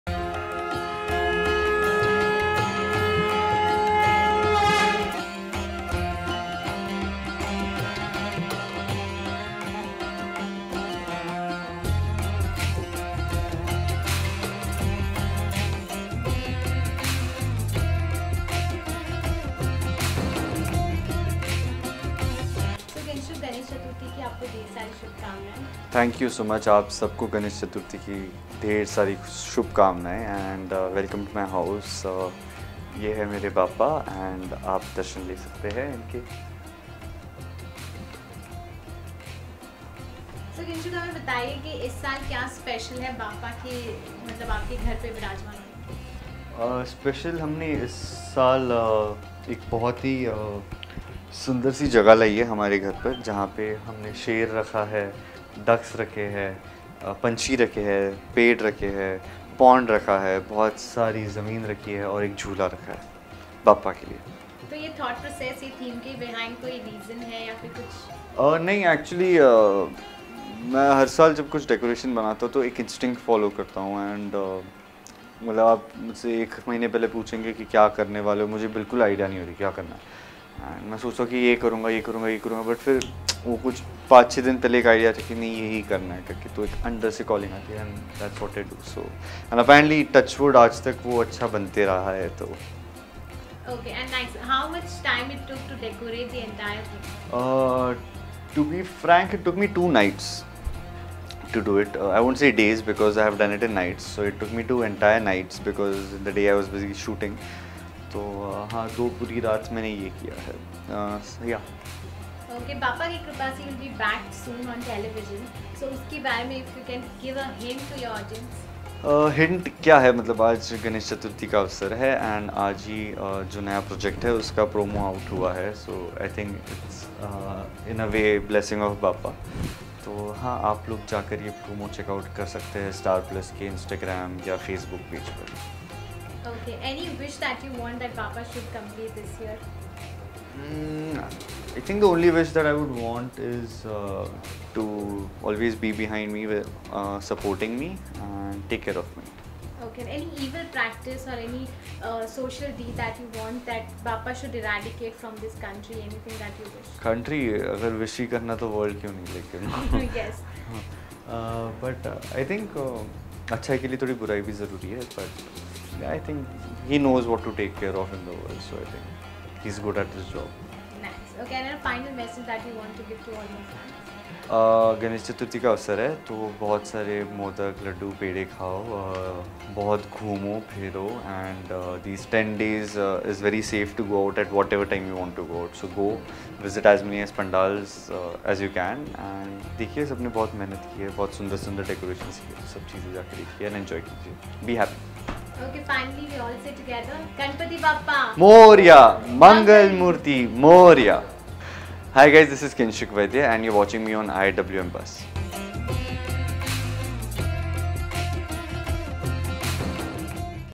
Ganesh Chaturthi celebration with Kinshuk Vaidya
Actor Kinshuk Vaidya celebrates Ganesh Chaturthi with IWMBuzz. Check out Kinshuk’s beautiful Ganpati décor, theme and more…

IWMBuzz is the preferred destination for news and updates from the world of Indian television and digital content ecosystem.